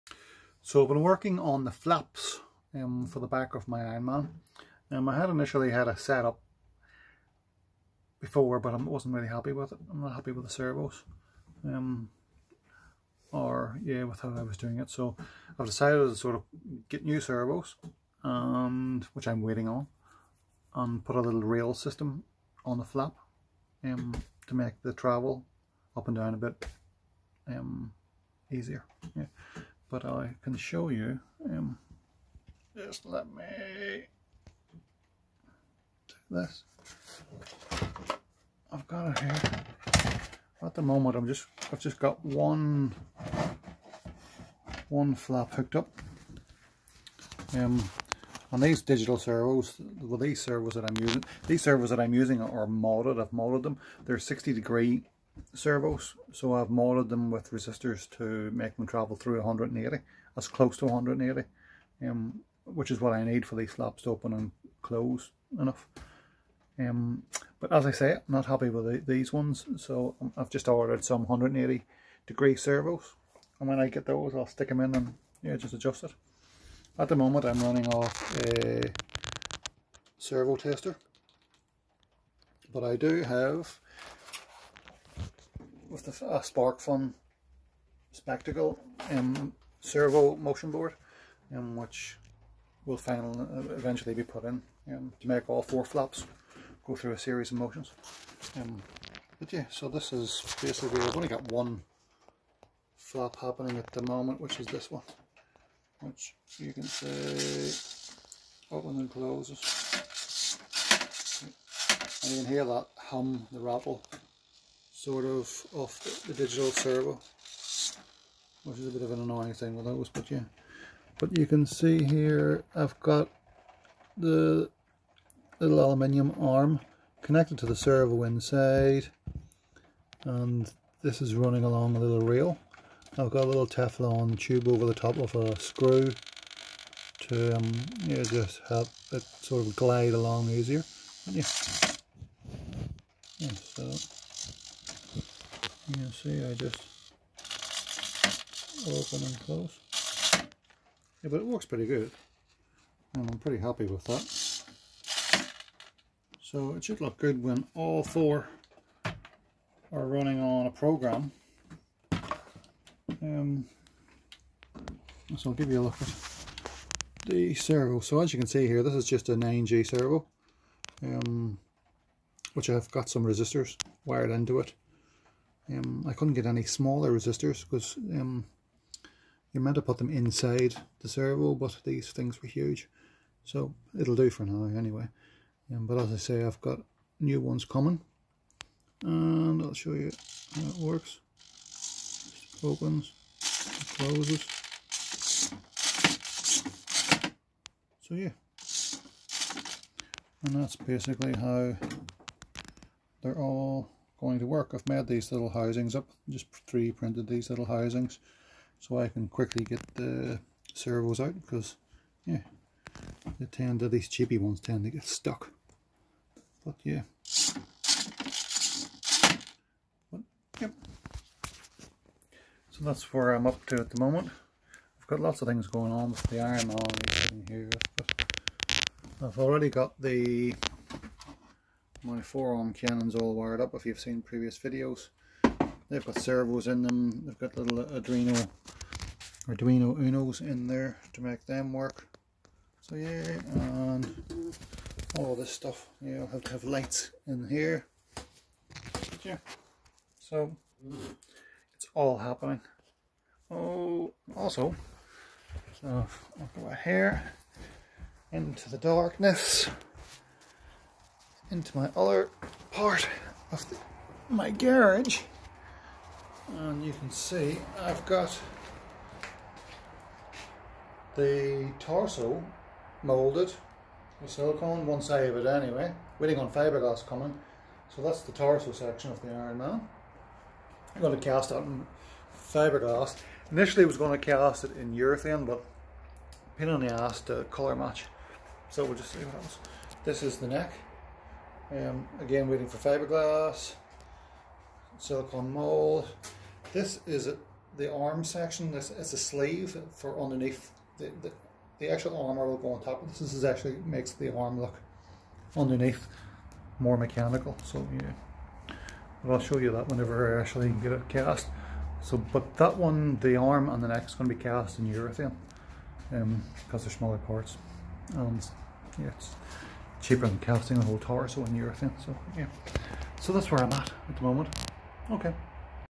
Ironman mk45 servo flaps
Getting back into this after a spinal injury so been a while. Originally used moded 60 degree servos so they'd travel 180 but decided to just order some 180 servos, was just tinkering with servos I already had. Will eventually run off a sparkfun spectacle motion board and will go through to series of motions, one a warm up opening and closing and second a fully opened 4 flaps hopefully with a jet smoke exhaust,  that's the plan.